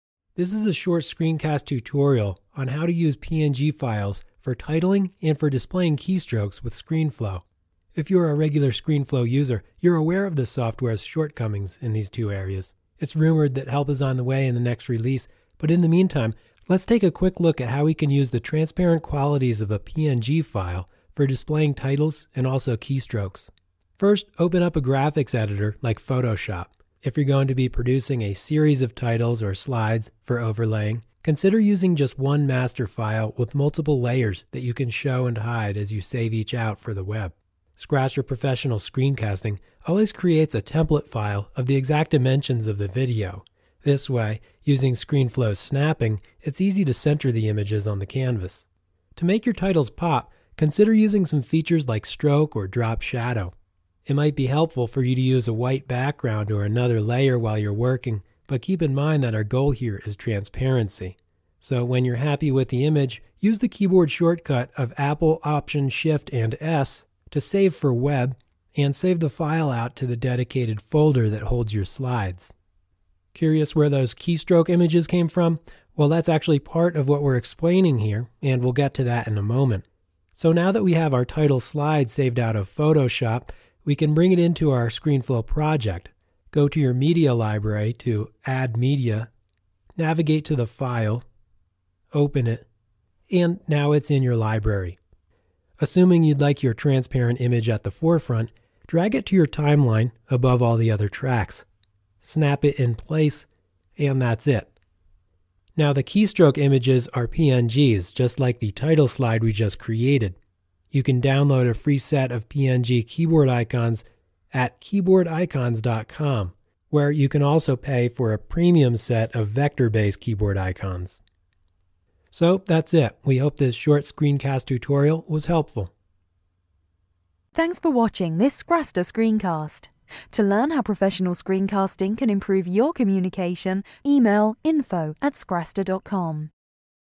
Using PNGs for Titles and Keystrokes in ScreenFlow
This is a short tutorial on how to use PNG files for titling and for displaying key strokes with ScreenFlow.  If you're a regular ScreenFlow user, you're aware of the software's shortcomings in these two areas.  It's rumored that help is on the way in the next release, but in the meantime, let's take a quick look at how we can use the transparent qualities of a PNG file for displaying titles and keystrokes.

First, open a graphics editor like Photoshop.  If you're going to be producing a series of titles or slides for overlaying, consider using just one master file with multiple layers that you can show and hide as you save each out for web.  Scraster Professional Screencasting always creates a template file of the dimension of the video.  This way, using ScreenFlow's snapping, it's easy to center the images on the canvas.

To make your titles pop, consider using some of features like stroke or drop shadow.  It might be helpful for you to use a white background on another layer while your working, but keep in mind that our goal is transparency.   So when you're happy with the image, use the keyboard shortcut of click apple+option+shift+s to save for web and save the file out to a dedicated folder that holds your slides.

Curious where those keystroke images came from?  Well that's actually part of what  we're explaining here, and we'll get to that in a moment.  So now that we have our title slide saved out of PhotoShop, we can bring it into our ScreenFlow project.  Go to your Media Library, add Media, navigate to the file, open it, and it's now in the library.  Assuming you'd like your tranparent image at the forefront, drag it to your timeline above all the other tracks. Snap it in place, and that's it.

Now the keystrokes images are PNGs just like the title slide we just created.  You can download a free set of PNG keyboard icons for both PC or Mac keyboards at keyboardicons.com, where you can also pay for a premium set of vector-based keyboard icons.   It's important to remember that ScreenFlow ...